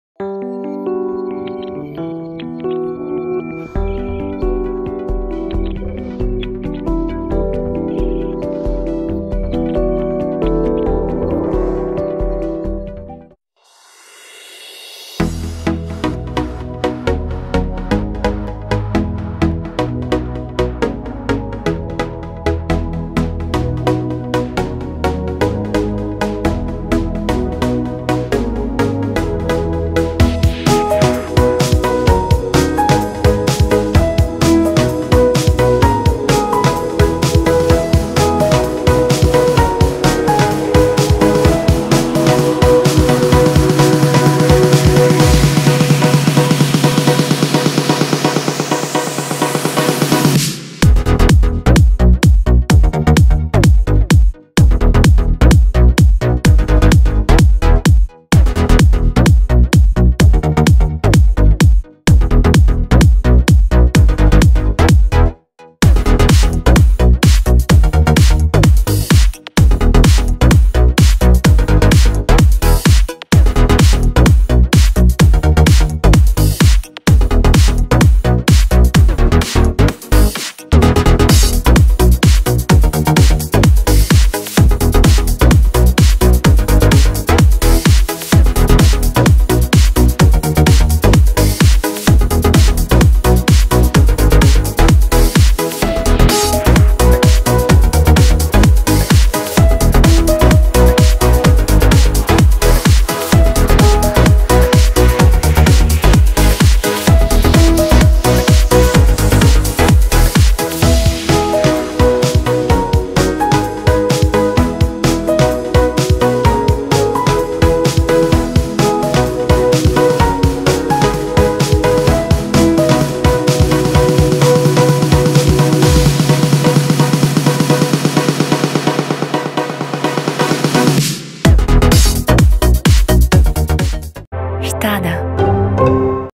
Штада Армения - Международная конференция гинекологов по теме Невынашивание бераменности 30.01.2016
Штада Армения
Международная конференция гинекологов по теме Невынашивание бераменности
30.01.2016